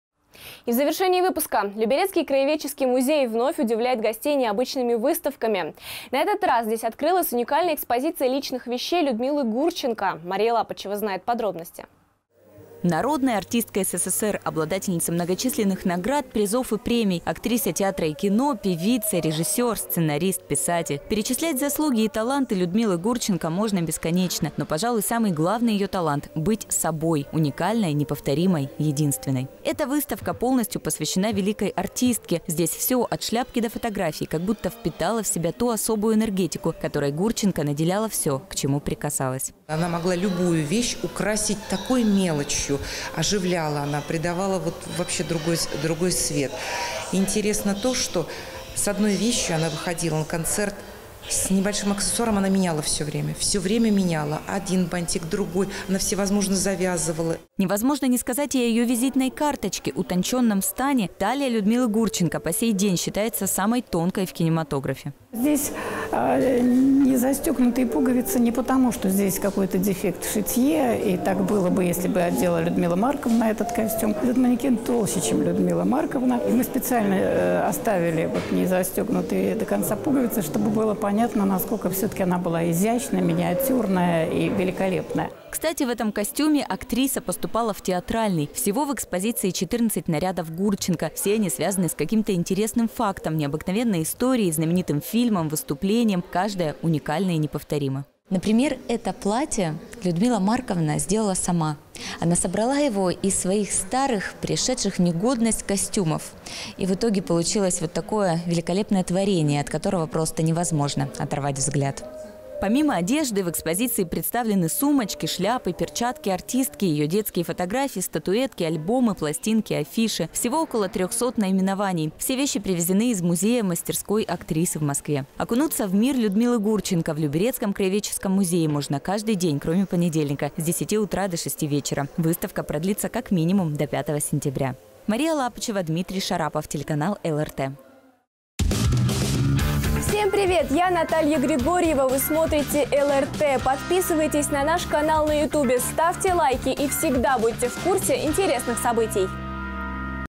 В Краеведческом музее открылась выставка вещей Людмилы Гурченко
Краеведческий люберецкий музей вновь удивляет гостей необычными выставками. На этот раз здесь открылась уникальная  экспозиция личных вещей Людмилы Гурченко. Мария Лапычева знает подробности.

Народная артистка СССР, обладательница многочисленных наград, призов и премий, актриса театра и кино, певица, режиссёр, сценарист, писатель... Перечислять заслуги и таланты Людмилы Гурченко можно бесконечно. Но, пожалуй, самый главный ее талант – быть собой: уникальной, неповторимой, единственной. Эта выставка полностью посвящена великой артистке. Здесь все – от шляпки до фотографии – как будто впитало в себя ту особую энергетику, которой Гурченко наделяла все, к чему прикасалась.

Ольга Засыпкина, заведующая отделом развития Люберецкого краеведческого музея:
"Она могла любую вещь украсить такой мелочью, оживляла она, придавала вообще, другой свет... Интересно то, что с одной вещью она выходила на концерт, с небольшим аксессуаром. Она меняла все время. Один бантик, другой. Она всевозможно завязывала".

Невозможно не сказать о ее визитной карточке – утончённом стане. Талия Людмилы Гурченко по сей день считается самой тонкой в кинематографе.

Татьяна Гордеева, работник Люберецкого краеведческого музея:
"Ну, здесь не застегнутые пуговицы не потому, что здесь какой-то дефект в шитье и так было бы если бы одела Людмила Марковна этот костюм. Этот манекен толще, чем Людмила Марковна. И мы специально оставили не застёгнутые до конца пуговицы, чтобы было понятно, насколько она была изящная, миниатюрная и великолепная".

Кстати, в этом костюме актриса поступала в театральный. Всего в экспозиции – 14 нарядов Гурченко. Все они связаны с каким-то интересным фактом, необыкновенной историей, знаменитым фильмом, выступлением. Каждое – уникально и неповторимо.

Мария Лапычева, корреспондент:
"Например, это платье Людмила Марковна сделала сама. Она собрала его своих старых, пришедших  в негодность костюмов. В итоге получилось  такое великолепное творение, от которого просто невозможно оторвать взгляд".

Помимо одежды, в экспозиции представлены сумочки, шляпы, перчатки артистки, ее детские фотографии, статуэтки, альбомы, пластинки, афиши. Всего около трехсот наименований. Все вещи привезены из музея-мастерской актрисы в Москве. Окунуться в мир Людмилы Гурченко в Люберецком краеведческом музее можно каждый день кроме понедельника, с 10 утра до 6 вечера. Выставка продлится как минимум до 5 сентября.

Мария Лапычева, Дмитрий Шарапов